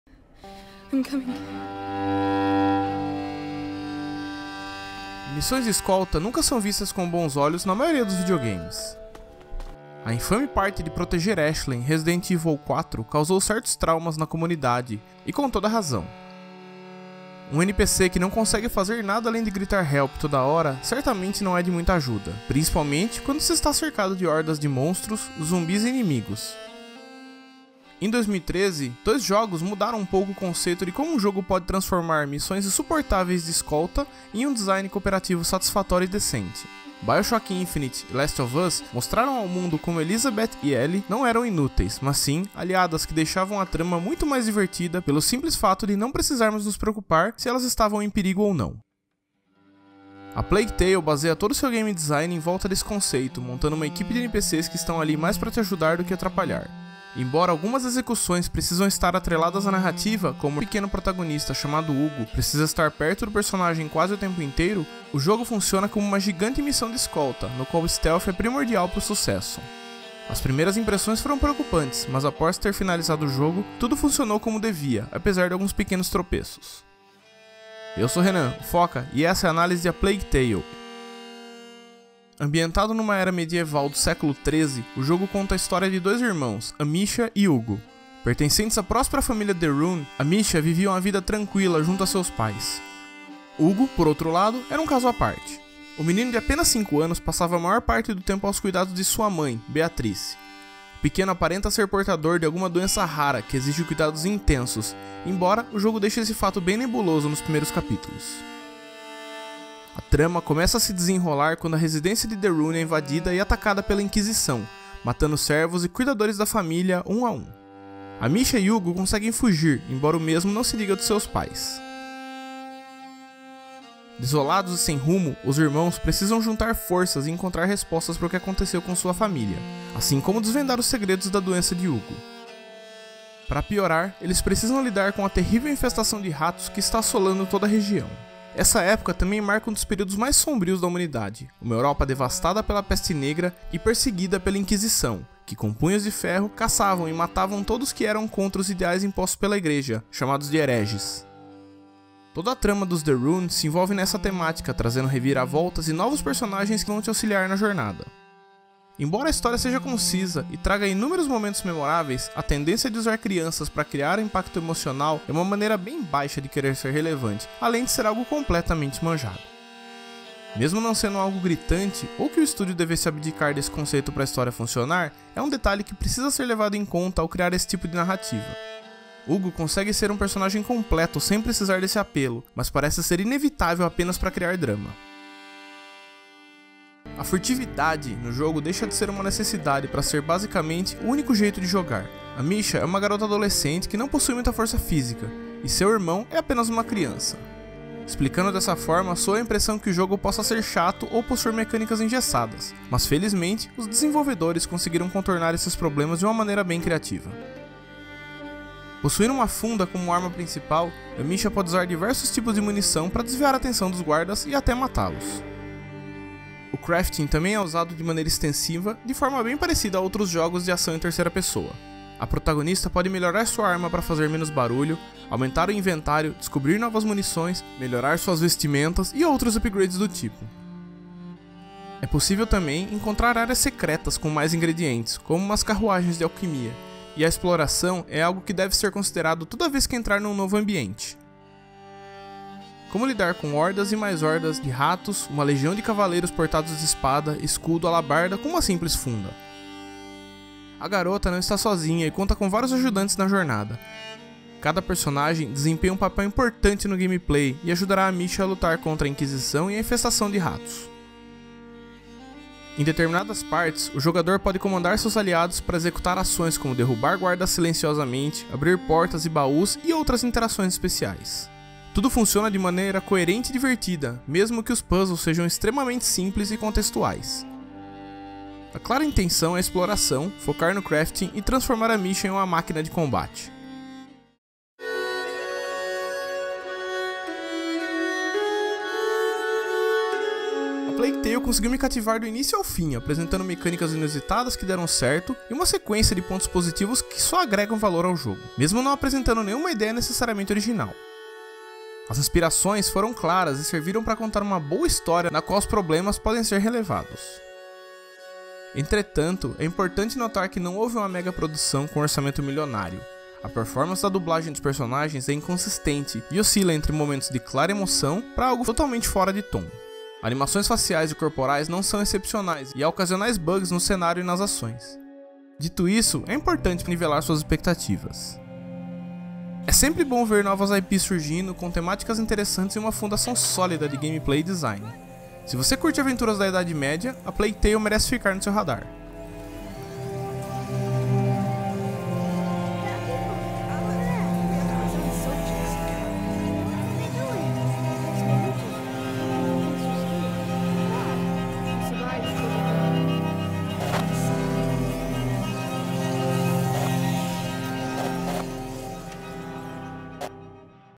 Por que Jogar A Plague Tale: Innocence? | Análise PS5, PS4, PC, Xbox | PS Plus Julho 2021
A Plague Tale: Innocence baseia todo seu game design em volta da mecânica de missões de escolta, montando uma equipe de NPCs que estão ali mais para te ajudar do que atrapalhar. Embora algumas execuções precisam estar atreladas a narrativa (como o irmão pequeno da protagonista, chamado Hugo, precisa estar perto da personagem quase o tempo inteiro), o jogo funciona como uma gigante missão de escolta no qual stealth é primordial para o sucesso.